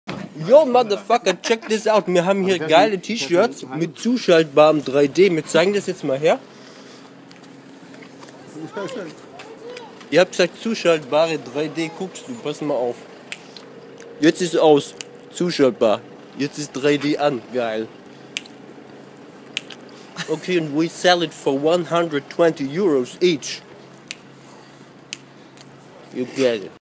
3D T-Shirt switchable / zuschaltbar
Worldwide first T-Shirt with switchable 3D...
Das weltweit einzige T-Shirt mit zuschaltbarem 3D.